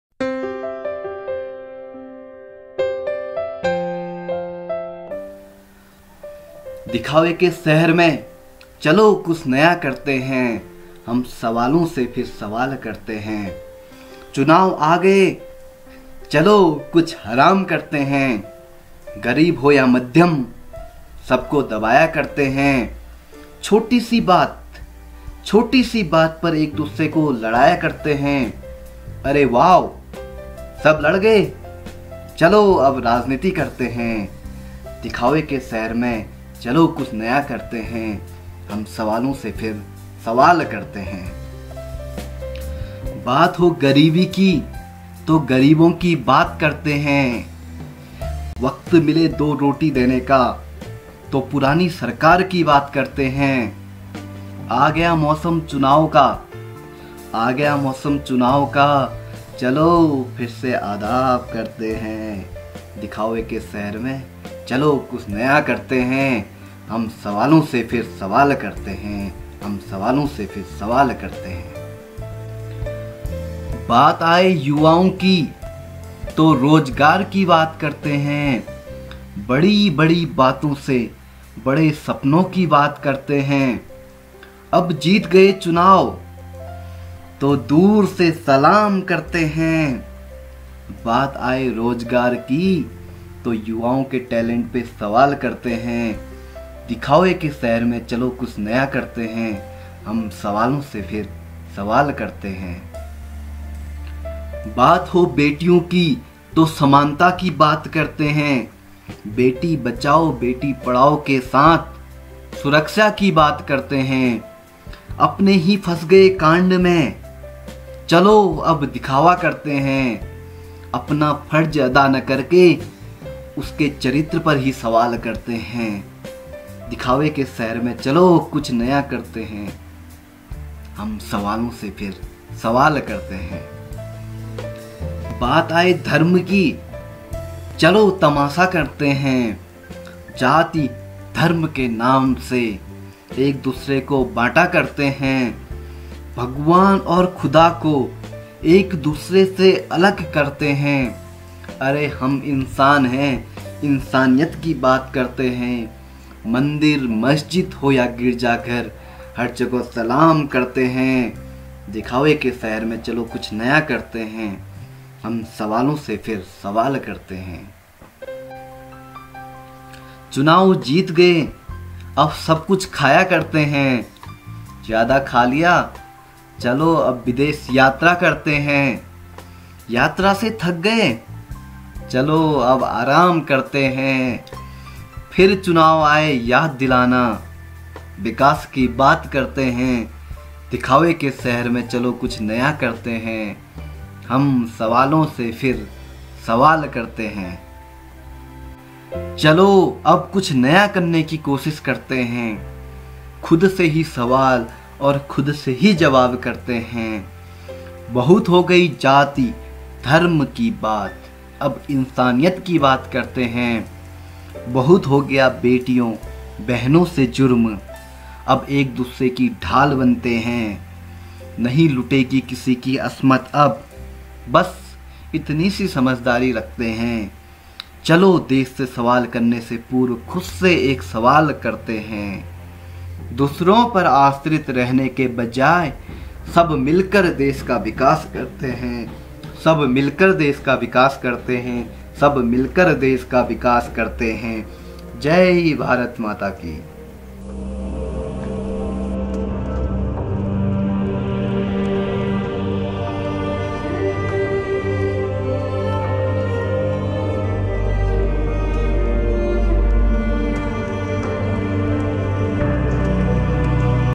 दिखावे के शहर मे चलो कुछ नया करते है || Santosh Tiwari
क्या यह कविता आज के देश की इस्थिति पर सटीक बैठती है???